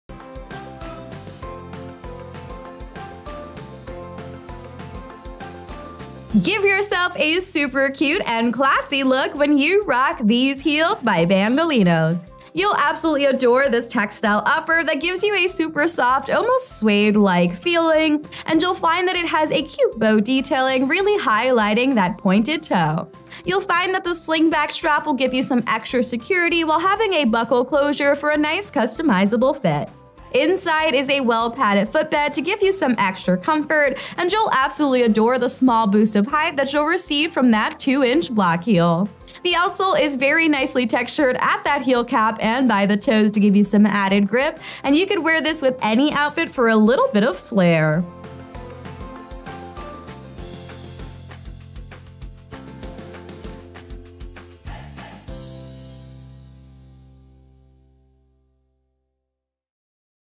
Bandolino Callo 2 SKU: 9781998
Be it your office hours or a fun night out with your friends, the  Bandolino® Callo 2 sandals is a perfect choice for your every outing.
Textile upper with bow detailing.
Textile lining with a cushioned footbed for lasting comfort.
Pointed toe silhouette.
Chunky block heel.
Adjustable buckle closure.
Durable synthetic outsole.
Imported.